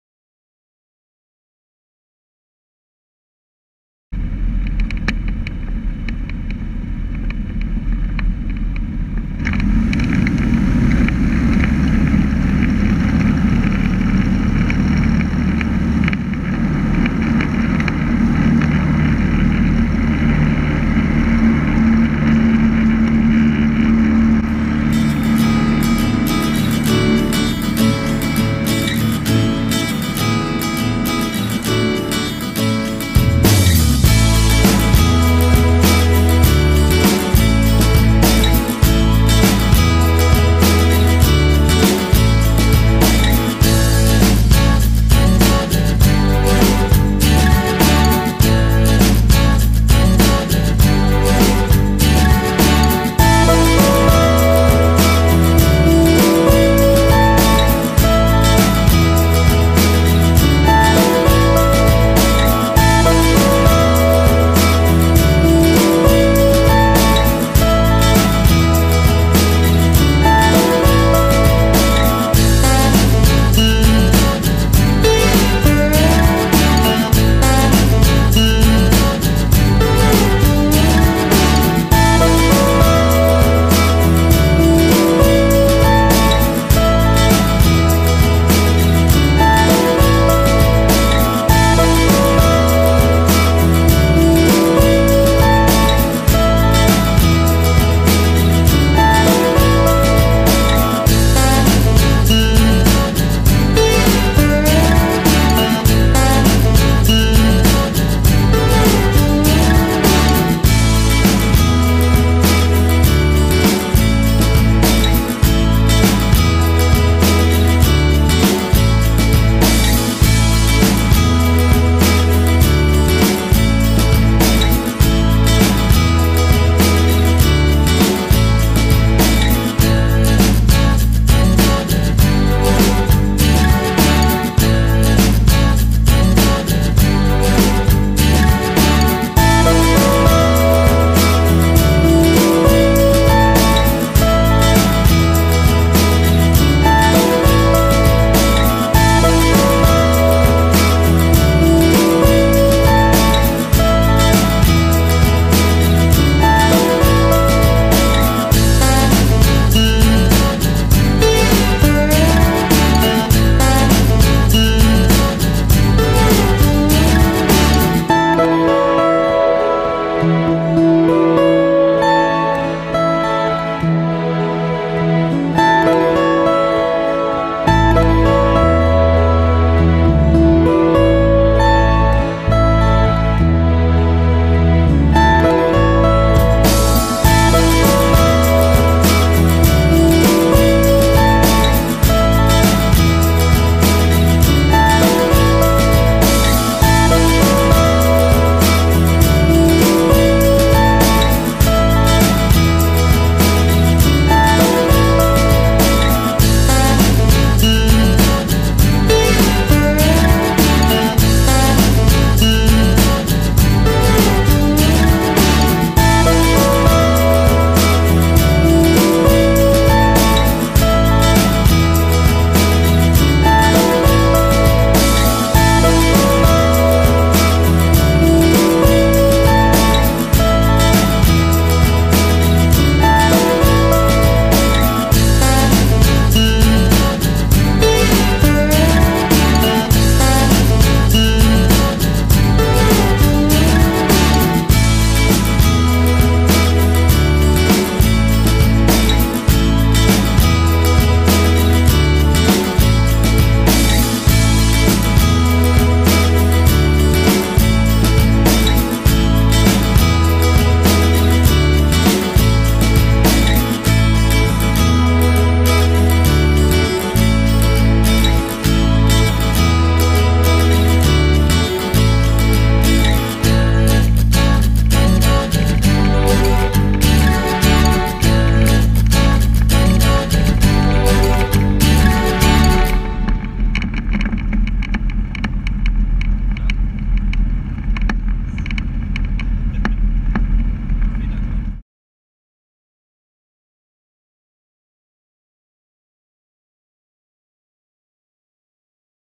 Cockpitview C42, flyvetur Stauning Venø
Pleasure flight in Denmark between Stauning Airport (EKVJ) and around the island of Venoe. Flown in a Comco-Ikarus C42 in March 2015.

Hyggetur imellem Stauning lufthavn (EKVJ) og rundt om øen Venø.
Fløj i en Comco-Ikaus C42, Marts 2015